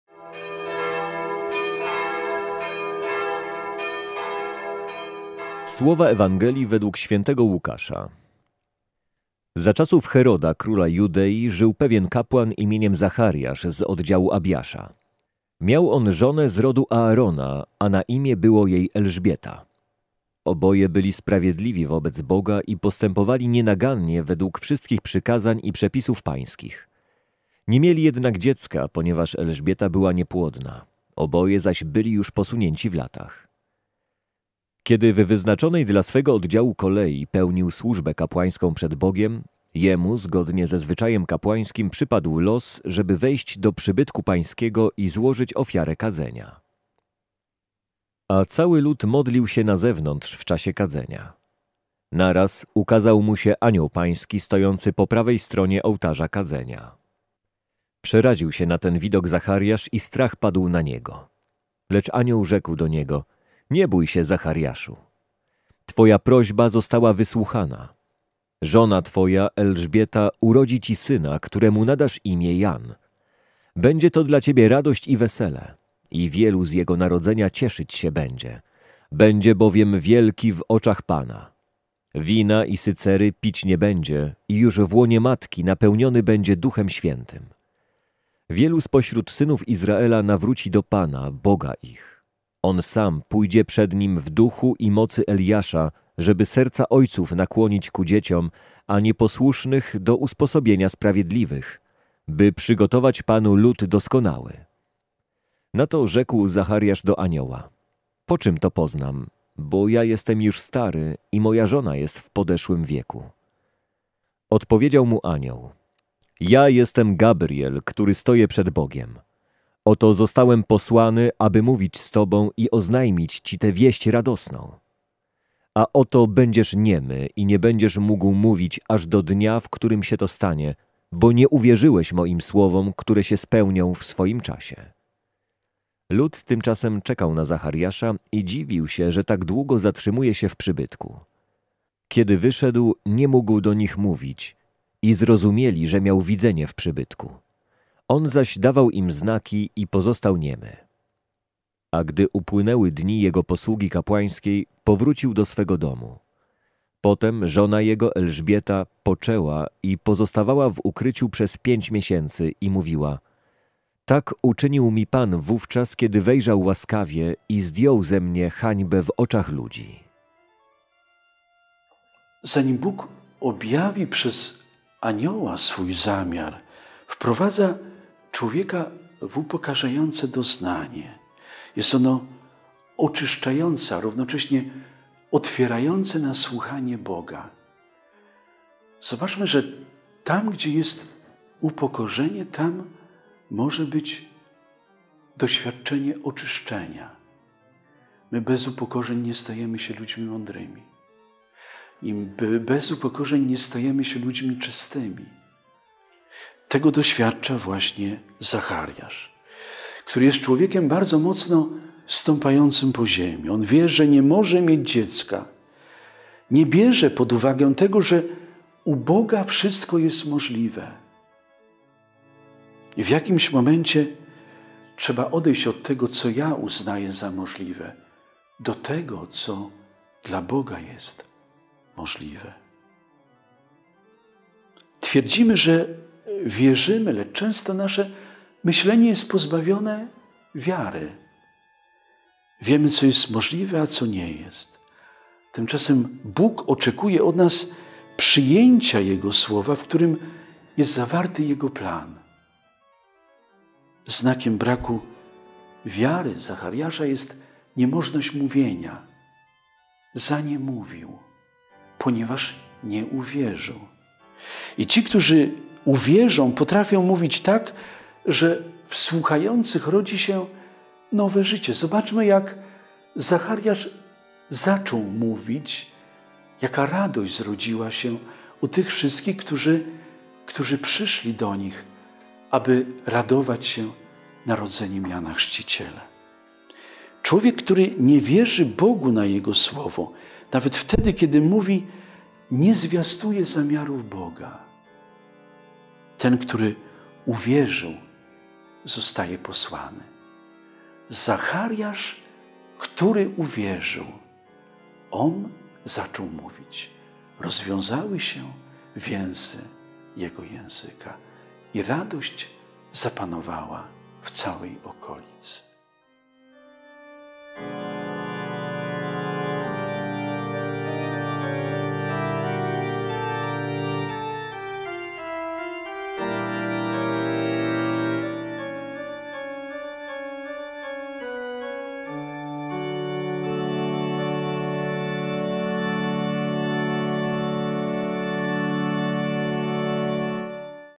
Dobra Nowina na dziś | 19 grudnia - Środa, III tygodnia adwentu
Medytację Ewangelii z dnia 19 grudnia prowadzi ks. Józef Pierzchalski SAC, spowiednik i uznany kierownik duchowy.